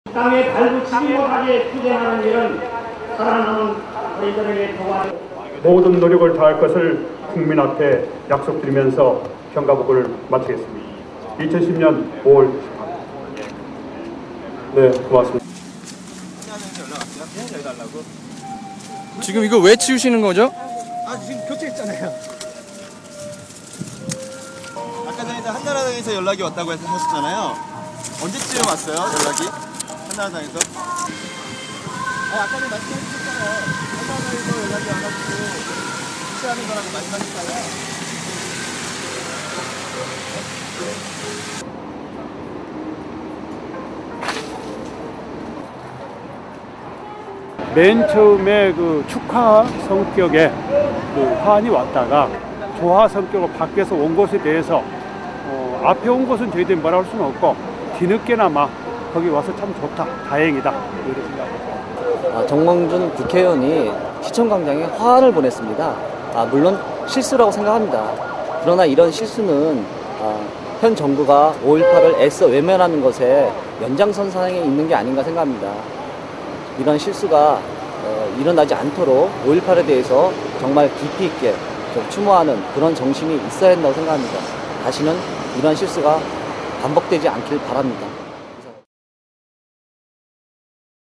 정몽준 대표 5·18 기념 축하 화환?
518 기념식장에 조화 대신 화환 보내 빈축
꽃집과 의사 소통 착오 해명 뒤 바꿔